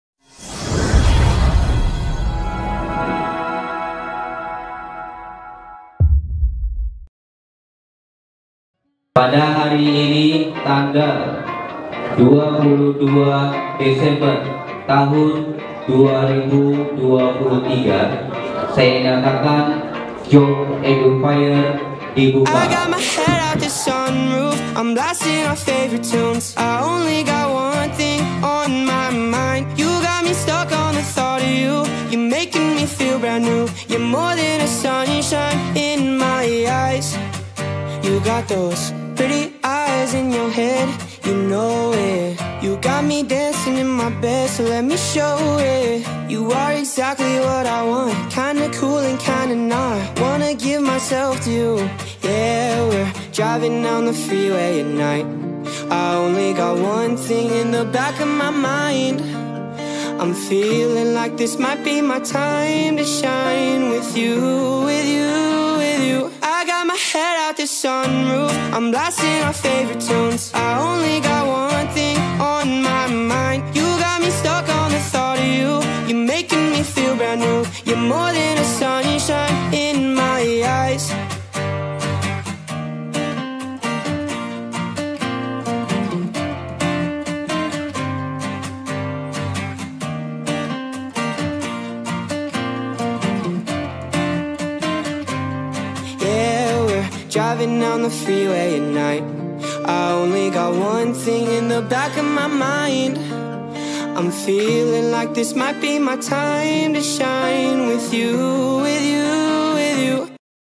SMK TI DWIGUNA Sukses! Gelar Job & Edu Fair - SMK TI DWIGUNA
Wargii..!!😍😍✨
Lulusan Sekolah Menengah Kejujuran (SMK) yang telah dibekali dengan pengetahuan dan keterampilan selama tiga tahun diharapkan segera mendapatkan pekerjaan atau melanjutkan Pendidikan ke perguruan tinggi yang diinginkan. SMK TI DWIGUNA Depok sebagai salah satu instansi Pendidikan berupaya memfasilitasi lulusan dan alumni untuk menentukan masa depan yang diimpiankan. Untuk pertama kalinya, SMK TI DWIGUNA Depok menggelar Job dan Edu Fair bertajuk “Break The Limit, Beyond Imagination” pada hari Jumat-Sabtu, 22-23 Desember 2023

Ibu Sri Hastuti, M.Pd. selaku ketua pelaksana kegiatan Job dan Edu Fair 2023 menyampaikan bahwa Job dan Edu Fair ini bertujuan untuk menjembatani siswa SMK TI DWIGUNA Depok dan alumni untuk bertemu dengan dunia usaha/dunia industri dan perguruan tinggi. Ia juga menyampaikan bahwa Job dan Edu Fair 2023 ini diikuti oleh tujuh dunia usaha/dunia indsustri dan empat perguruan tinggi.

Selain pembukaan stand yang diikuti oleh dunia usaha/dunia industri dan perguruan tinggi, dalam acara Job dan Edu Fair 2023 ini juga diadakan P5 "Unjuk Karya Siswa" sehingga keduanya akan saling berkaitan.

Harapannya semoga acara ini dapat memberikan manfaat dan kesempatan emas bagi para peserta. Mari jalin kemitraan yang baik antara sekolah, dunia kerja, dan perguruan tinggi. untuk menciptakan generasi yang siap menghadapi masa depan.